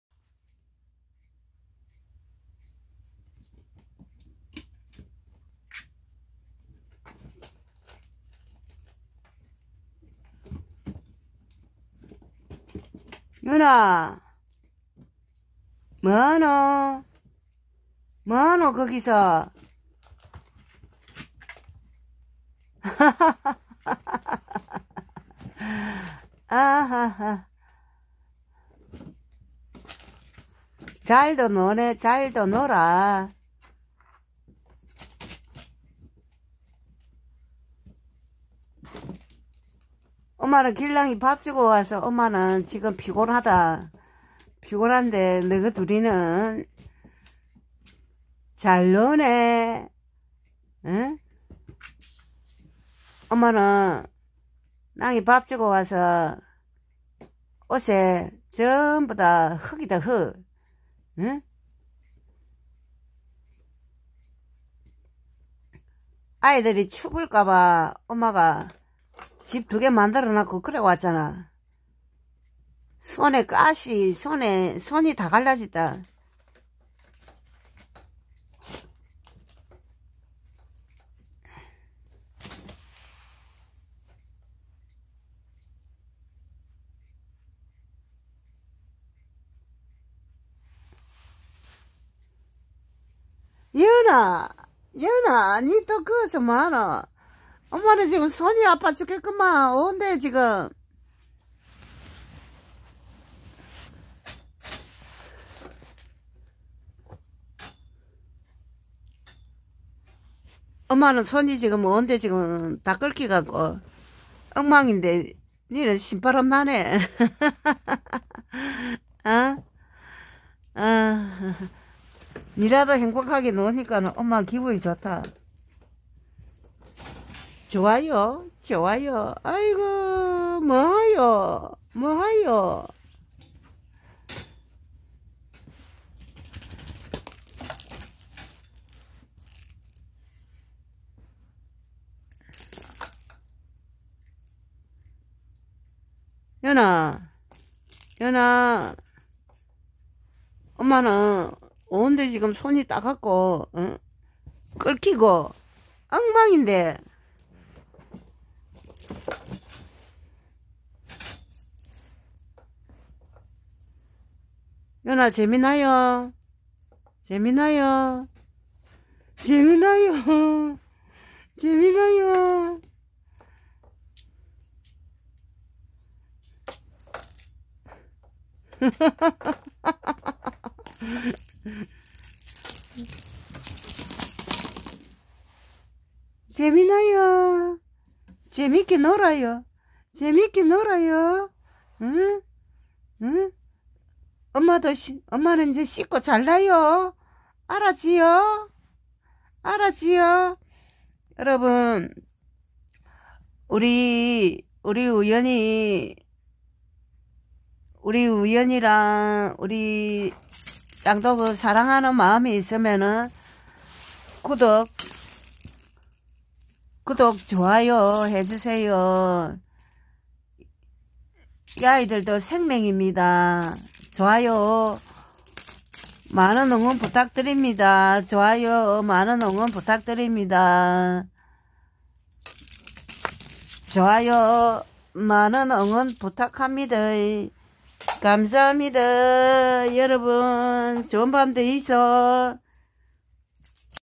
막둥이 우연이 뭐하요 ㅎ ㅎ #
안녕하세요 길냥이밥주고 집에 들어오니 남편은 주무시고 짱도그는 늦은밤 소리내어 짖고 우연이는 이리갔다 저리갔다 길냥이겨울추울까봐 나무와 낙엽으로 집을 만들어주고 와서요 마음이 가벼워요 항상 감사합니다 건강관리 잘하세요 고맙습니다 우정입니다